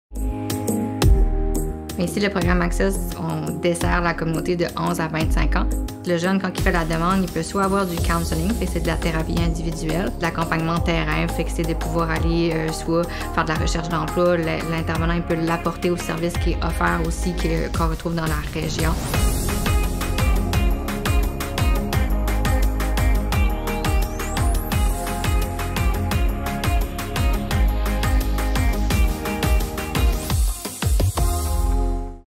ACCESS Open Minds - Meeting the Need
Meeting the Need - There's been a 60% increase in referrals of young people seeking help from the first year to the second year of operations.